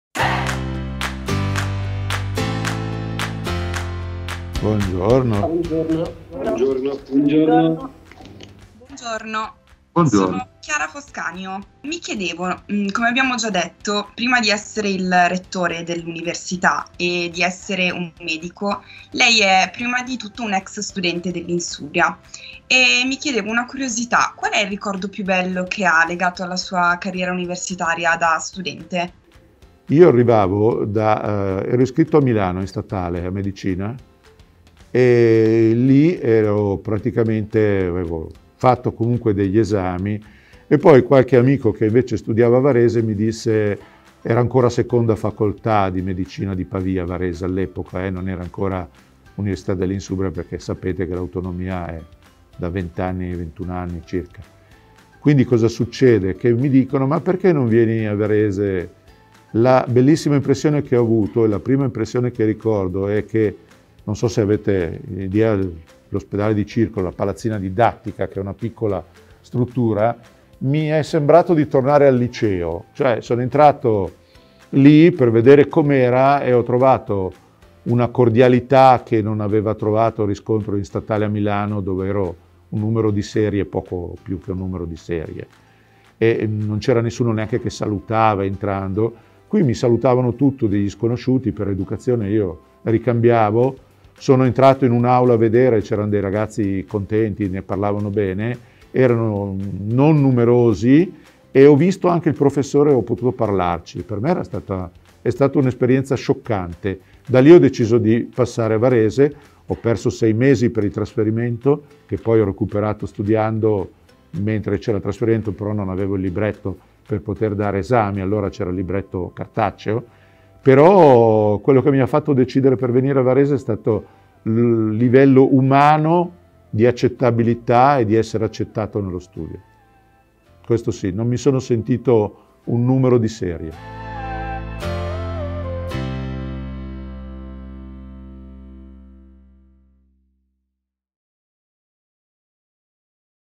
Mi è sembrato di tornare al liceo
Siate tenaci e sognatori: Il rettore intervistato dagli studenti